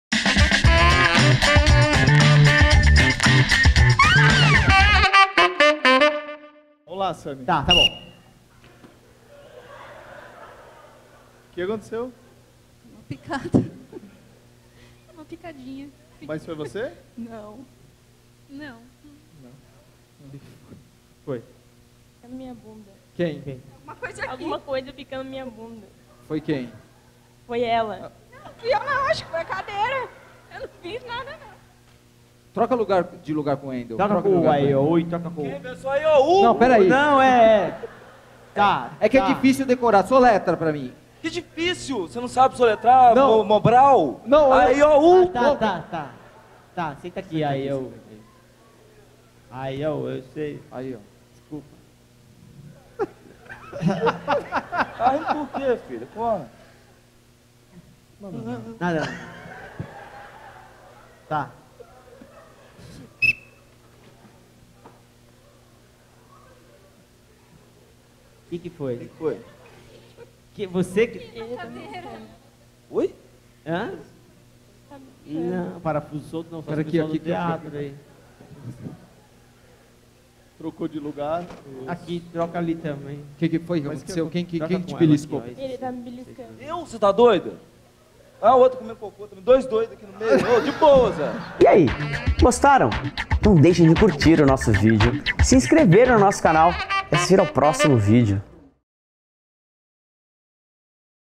SHOW DE HIPNOSE CÔMICA - BELISCADA NO BUMBUM (12/08/17)
Em nossa apresentação do dia 12/08, apresentamos nossa famosa "Beliscada no Bumbum"

*** TEATRO FOLHA ***
Local: SHOPPING PÁTIO HIGIENÓPOLIS
2017 - Setembro e Outubro - Todos Sábados às 23h59